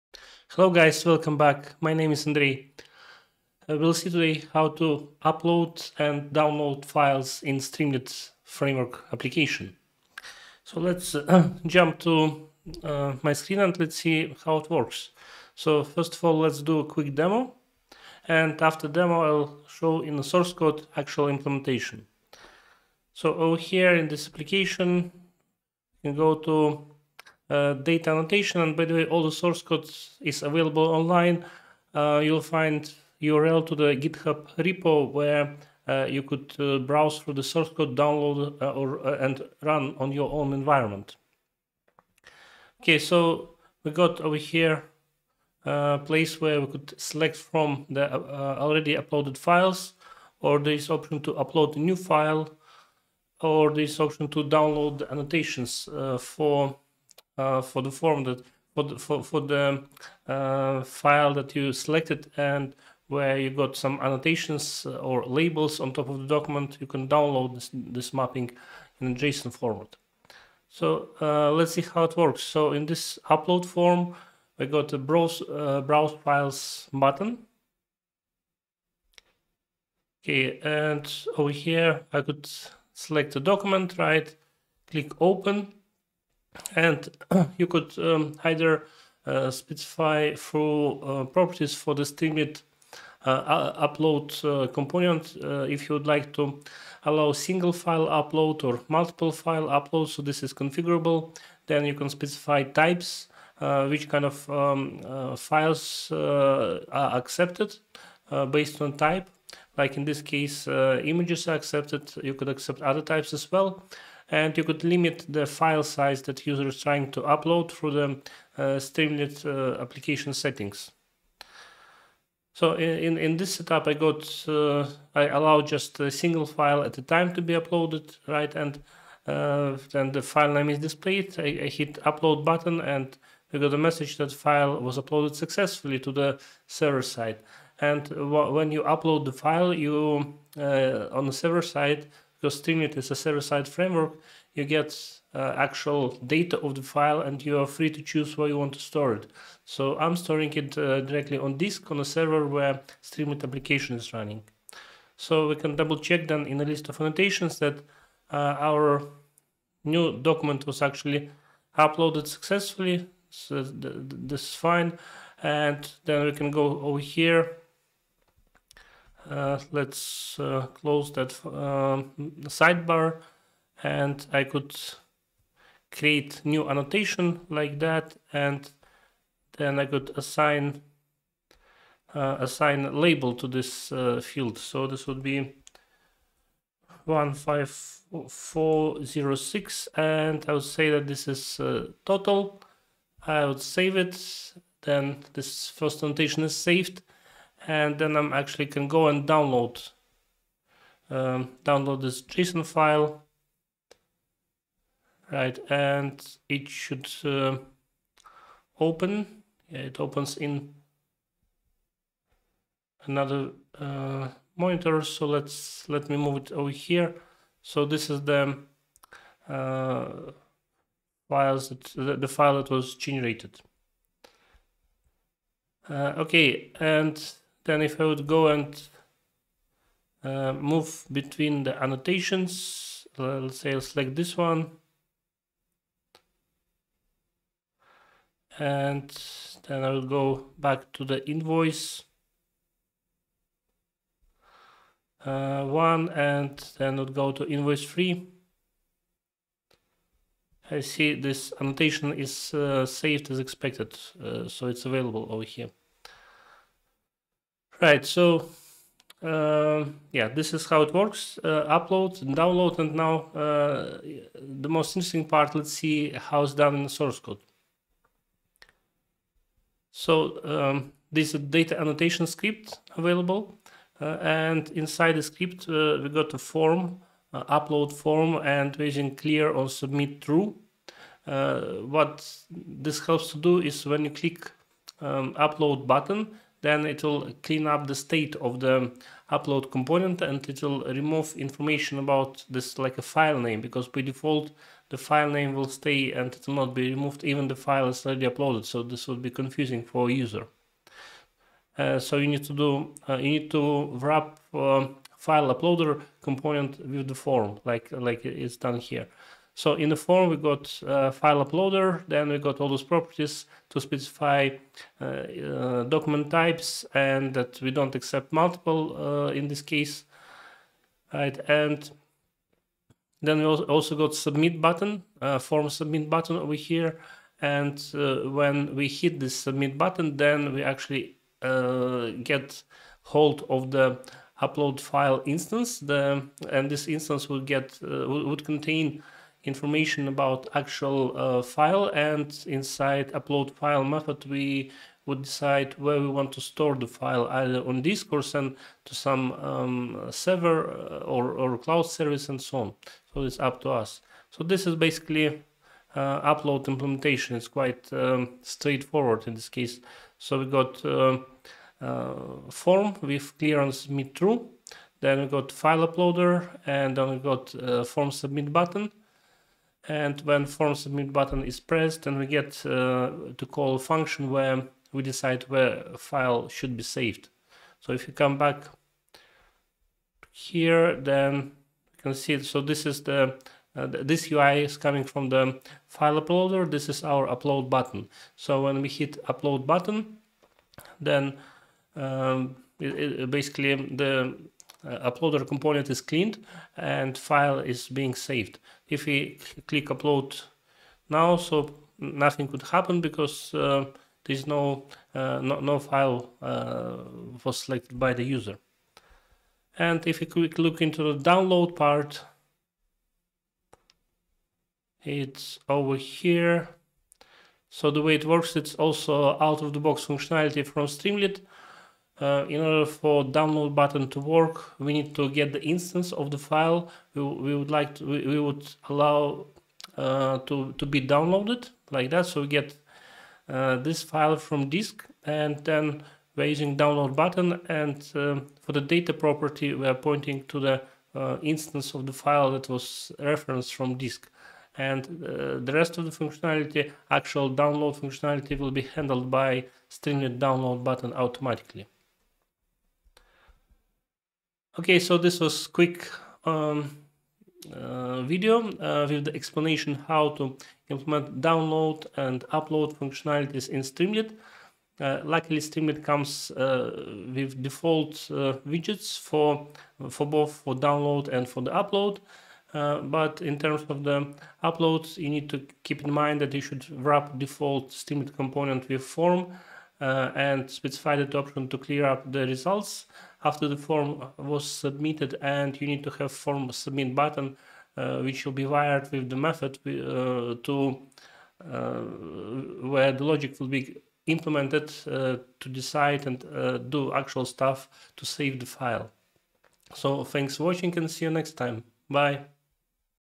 File Upload/Download in Streamlit/Python
File upload/download is supported by Streamlit out of the box. There are few hints to share about more effective file upload implementation. You will learn how to wrap file upload widget with Streamlit form, use Submit button to confirm the upload and reinitialize the upload widget. Additionally, I will show you an example of how to download JSON file from the server with Streamlit download component.

0:00 Introduction
0:12 Demo
4:25 File Upload code
6:58 File Download code
7:35 Summary